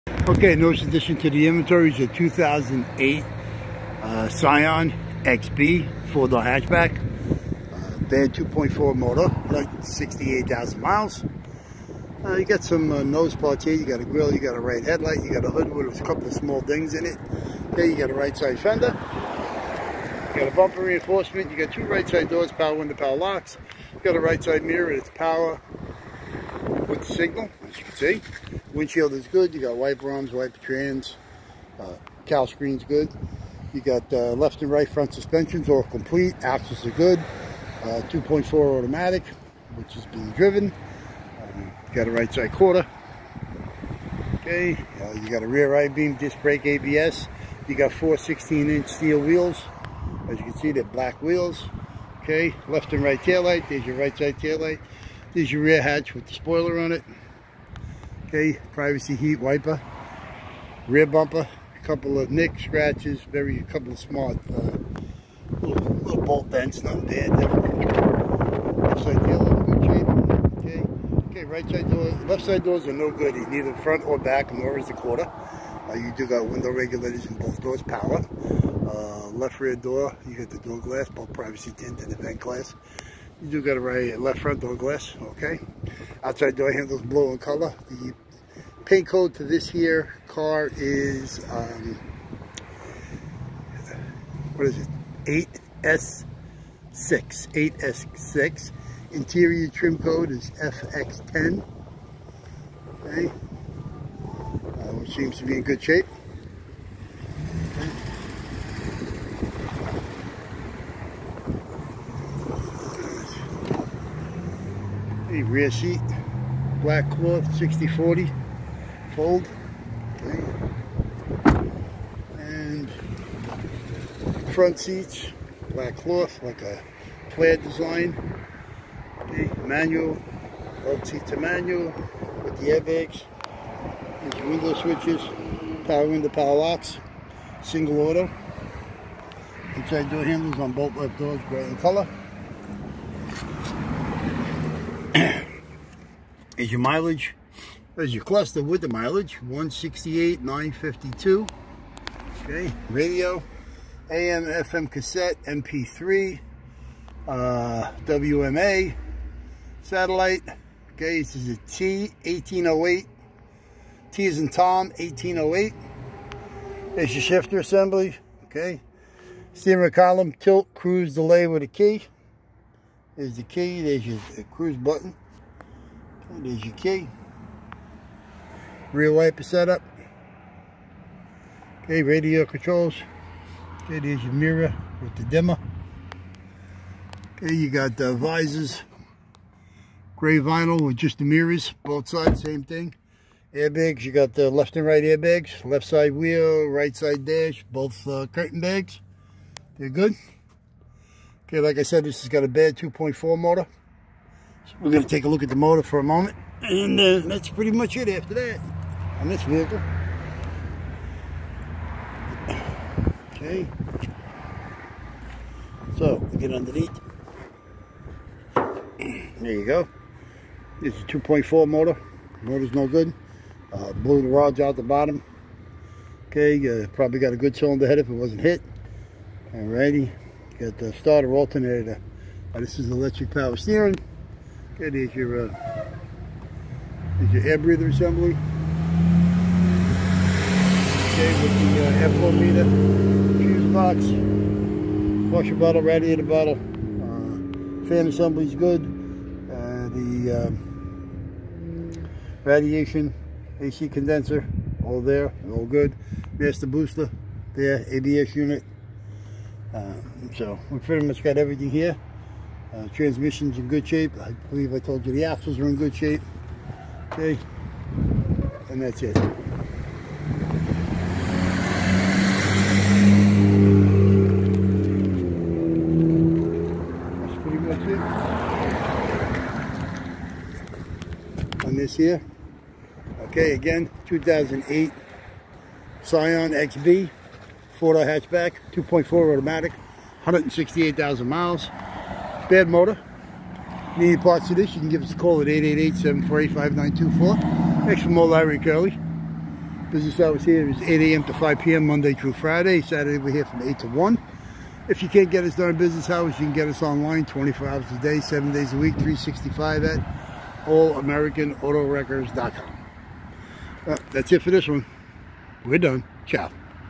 All American Auto Wreckers is Parting out this 2008 Scion XB 2.4 Automatic bad motor Stock # 8454
This 2008 XB came in with a bad 2.4 engine.  The automatic transmission is good and driven. Besides the engine problem the only other issue was the both left doors are damaged. we have a lot of good clean OEM body parts on this 2008 Scio XB. The bad engine still has good parts and we will stock a lot of these parts and have them ready for you. The front suspension has abs brakes and we have all of the parts. We have the disc brake rear I-beam complete also.  This 2008 XB also has front and rear seats in a black cloth. The exterior Paint Code is 8SL-Blue. The Interior Trim Code is FA10-Black Cloth. The Build date is 10/2007. So if Moe-Larry or Curly can help you with Used Auto parts from this m2008 Scion XB PLEASE call us Toll-Free at .